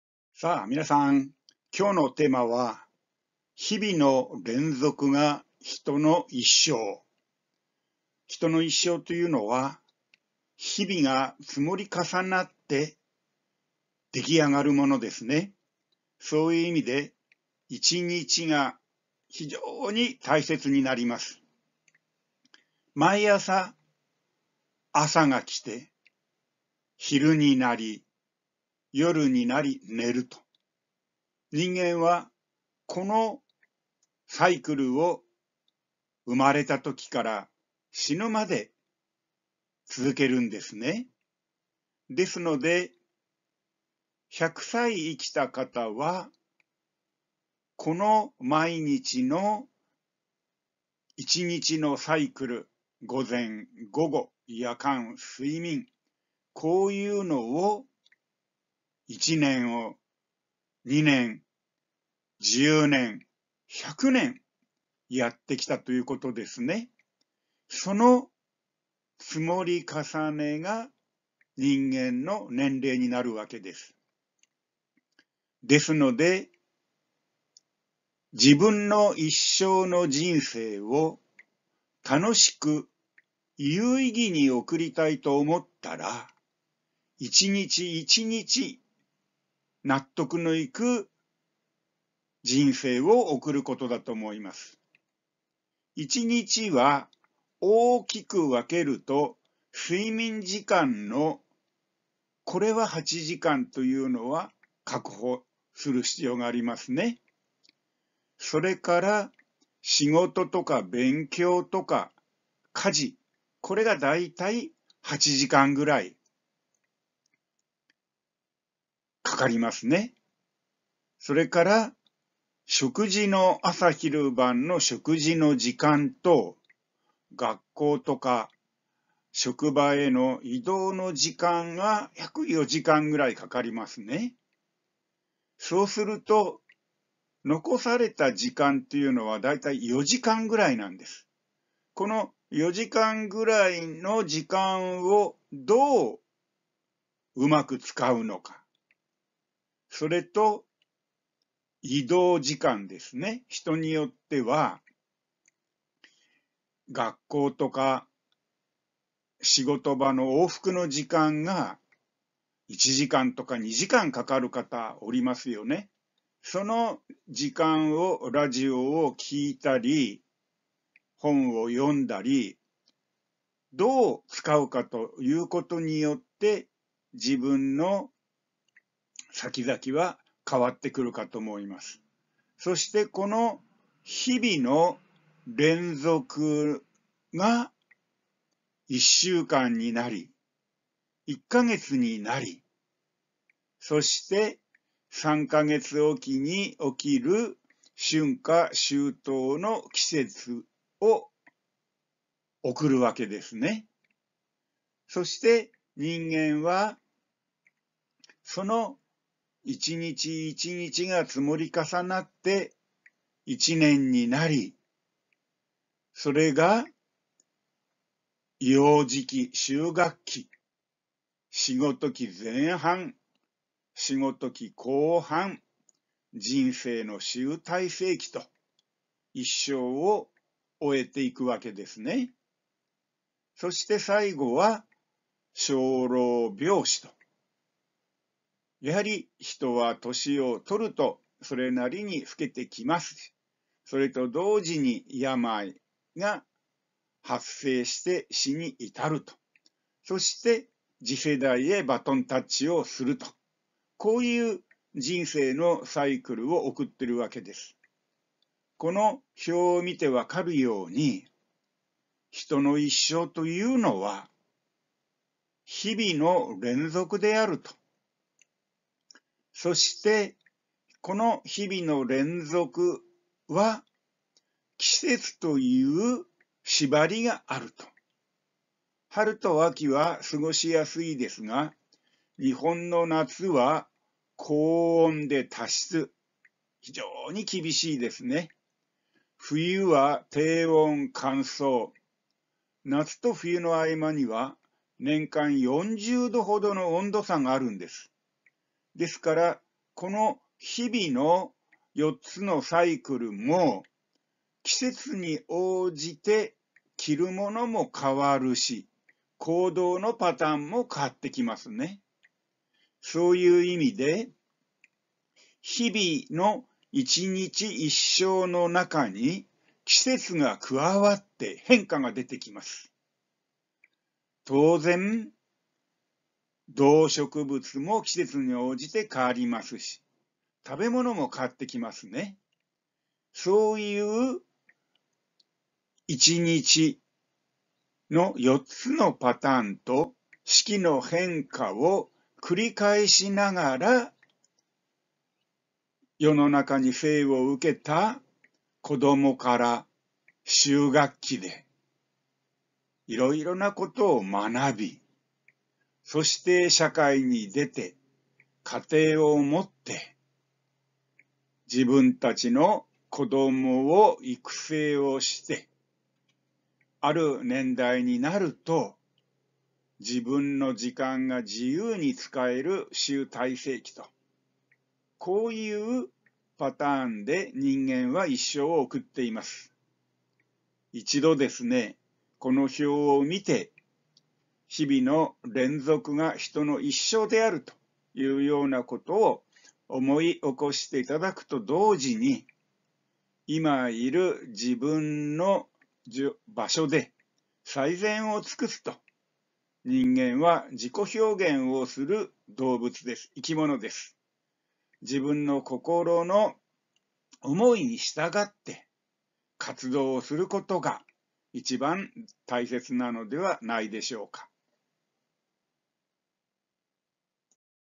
２０２０１２１４ 日々の連続が人の一生
人の一生は「一日一生」
日々の連続が人の一生です。

日々の時間を悔いのないように生きることが、
悔いのない一生に繋がります。